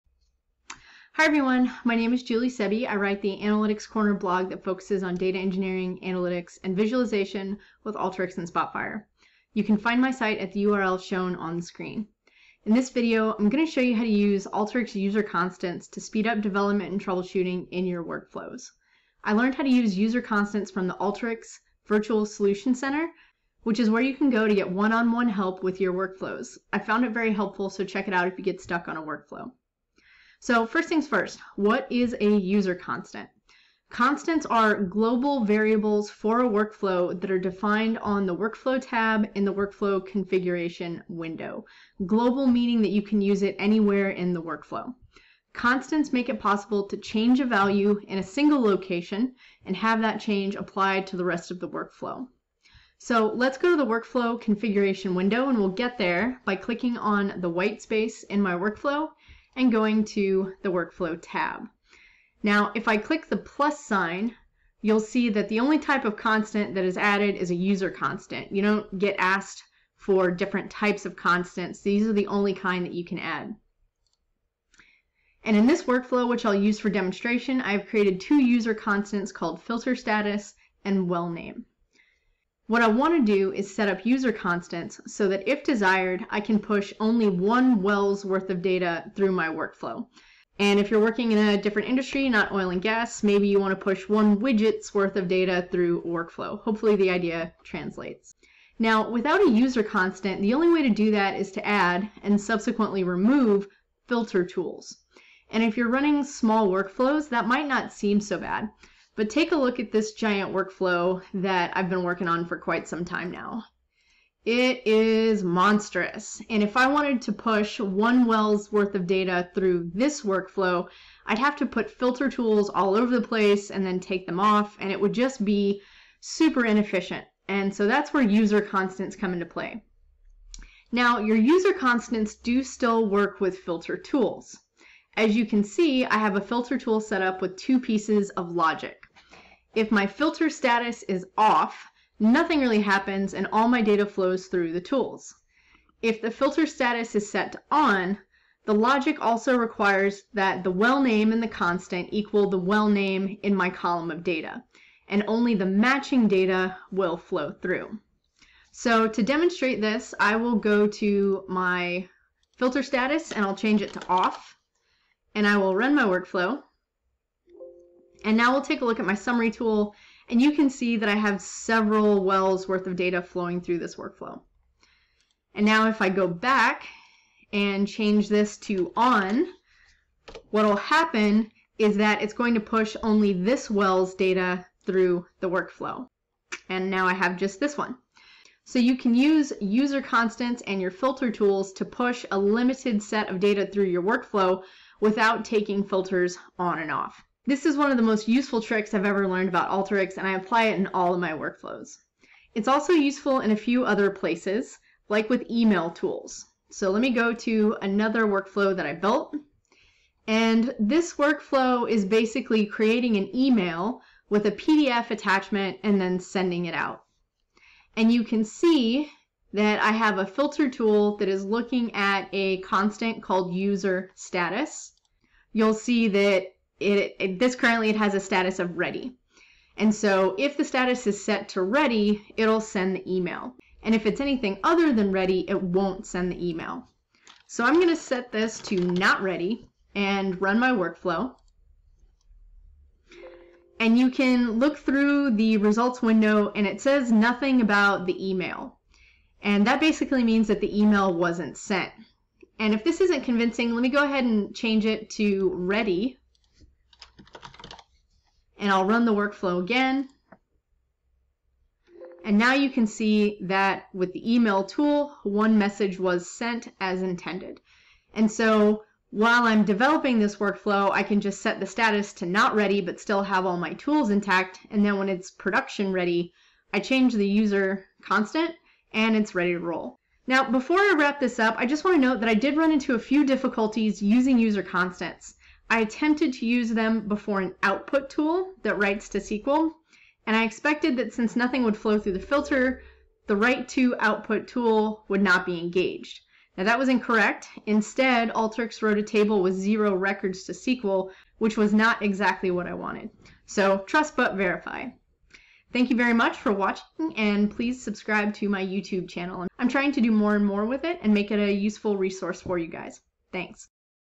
Alteryx - Accelerate Workflow Development with User Constants
In this video I am going to show you how  use Alteryx user constants to speed up development and troubleshooting in your workflows.  I learned how to use user constants from the Altery Virtual Solution Center, which is where you can go to get one on one help with your workflows.  I have found it very helpful, so check it out if you get stuck on a workflow.